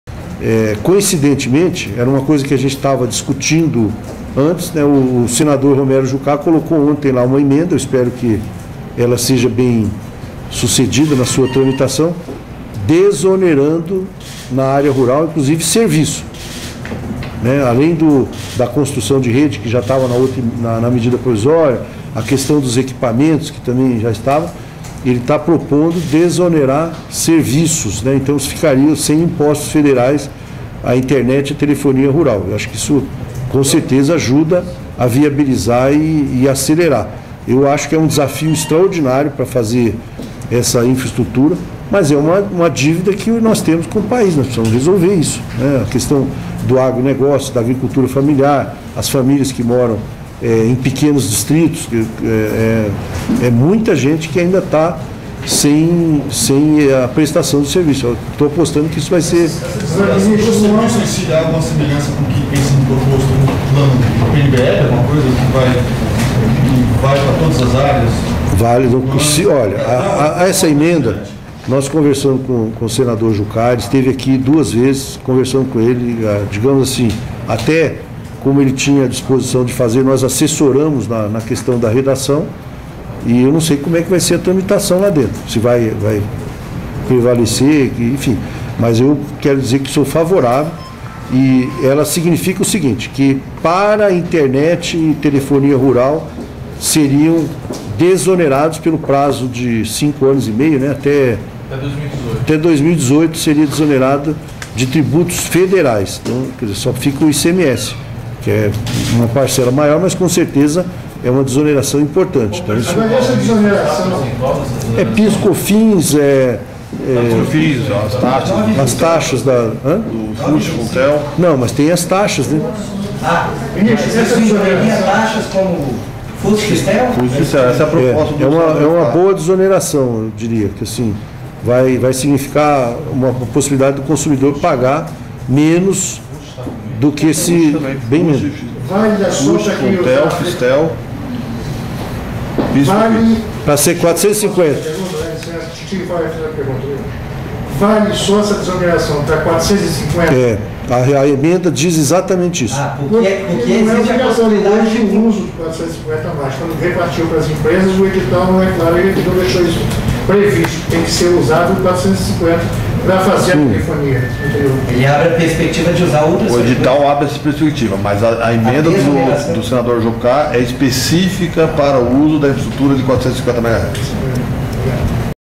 Governo elimina impostos para Internet e telefonia na área rural
Transformação da MP 563/2012 em Lei prevê isenção de todos os tributos federais, inclusive Fust e Fistel, em equipamentos e serviços oferecidos com a faixa de 450 MHz. Mesma proposta inclui smartphones e roteadores na Lei do Bem e ainda reduz Fistel para sistemas M2M. "Internet e telefonia rural ficarão sem impostos federais até 2018", diz o ministro das Comunicações, Paulo Bernardo.